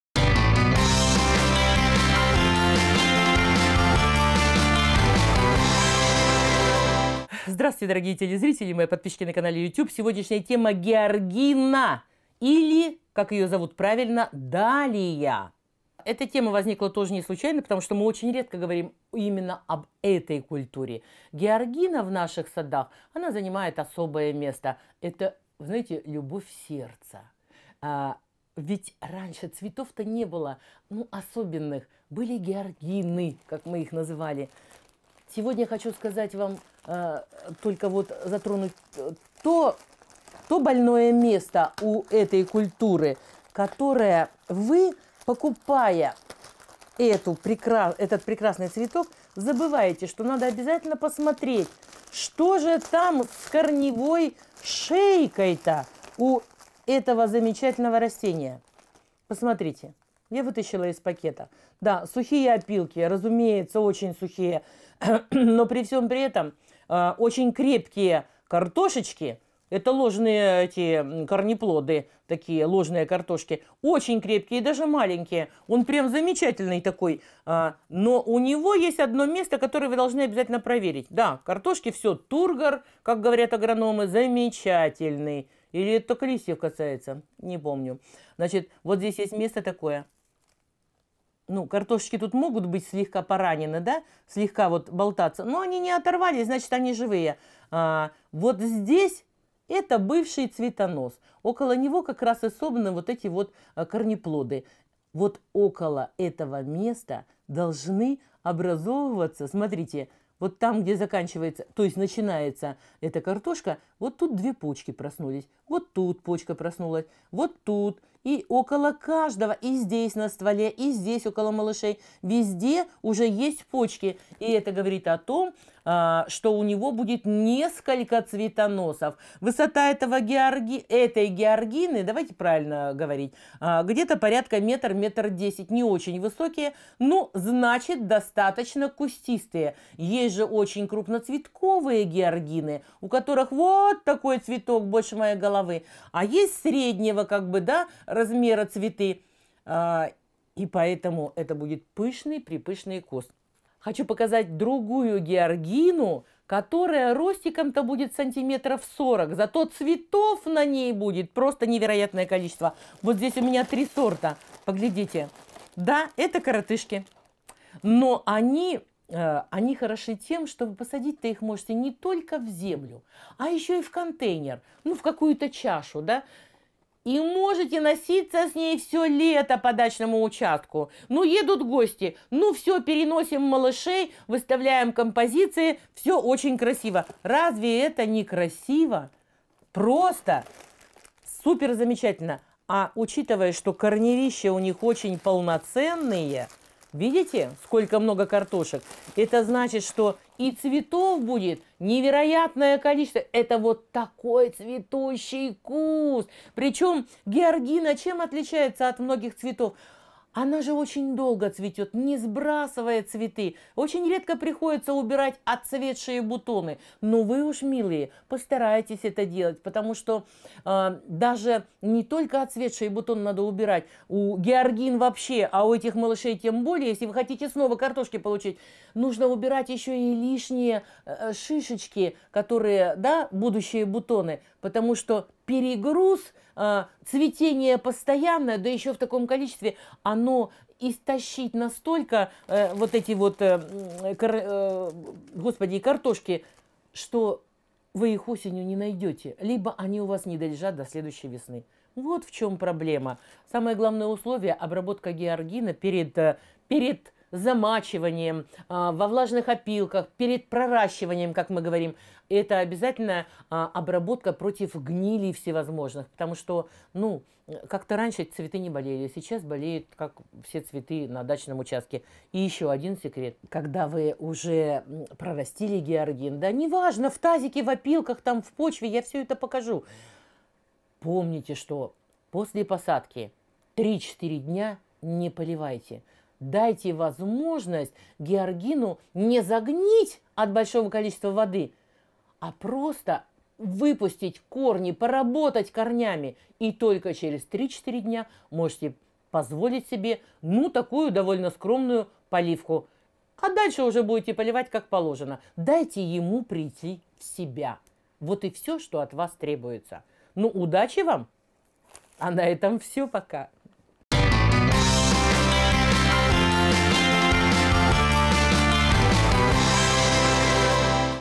Голова садовая - ВАЖНО! Как выбрать посадочный материал ГЕОРГИН
В этом выпуске расскажу, как выбрать посадочный материал георгин. Как не ошибиться при покупке и что выбрать, чтобы получить богатый красивый цветник на даче.